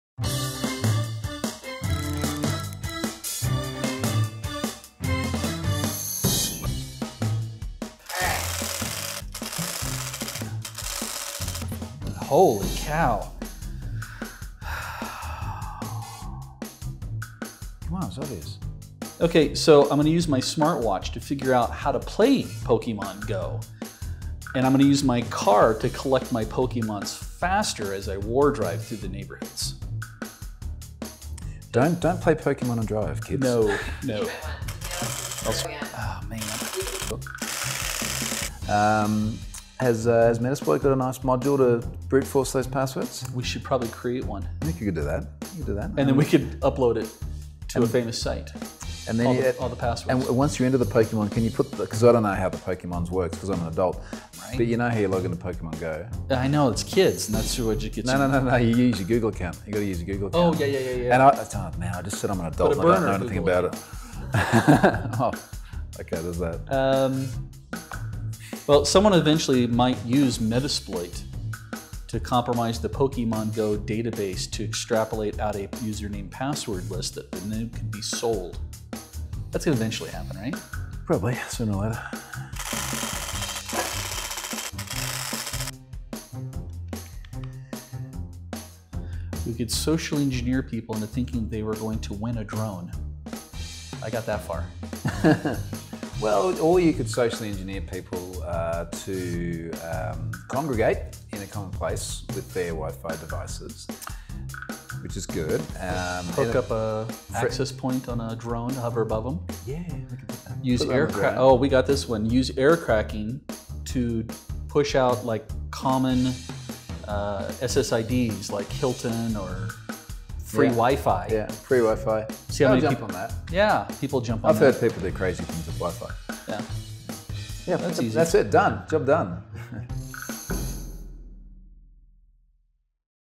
Pluralsight Hack Roulette with Dale Meredith and Troy Hunt
To celebrate the launch of our new information and cyber security training category, we invited authors and security bad boys Dale Meredith and Troy Hunt to our HQ for a little game of Hack Roulette.

Hack Roulette | Pluralsight